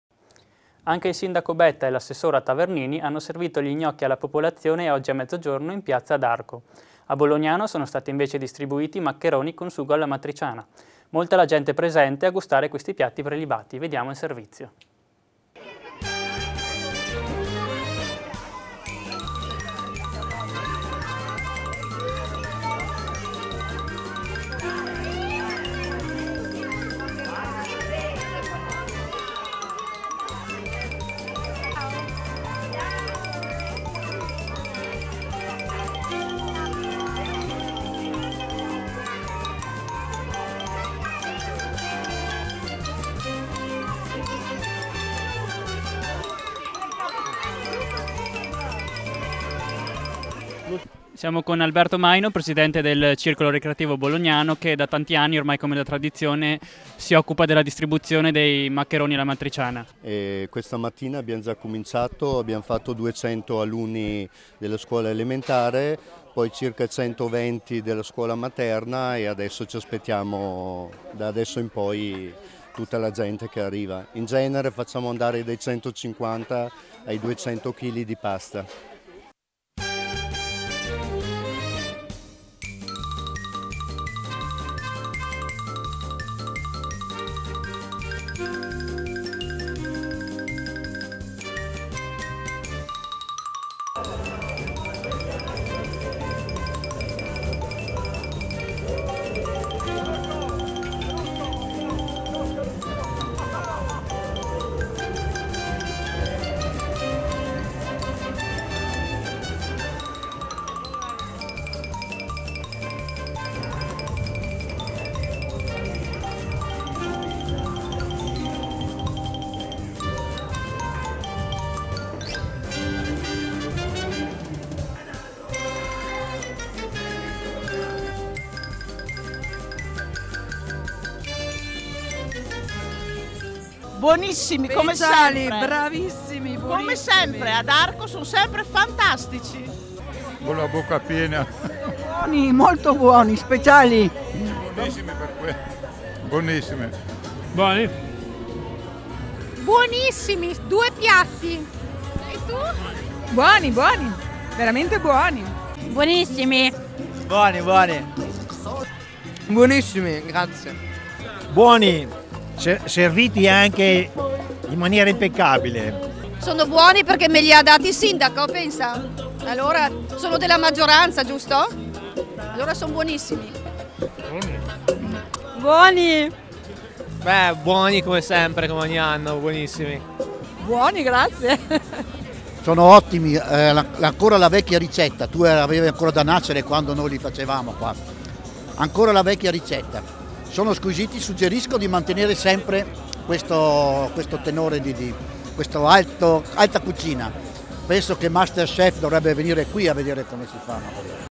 Distribuzione degli gnocchi ad Arco
Distribuzione gratuita degli gnocchi ad Arco grazie al Comitato Gnocchi Arco, servizio del 13 febbraio 2015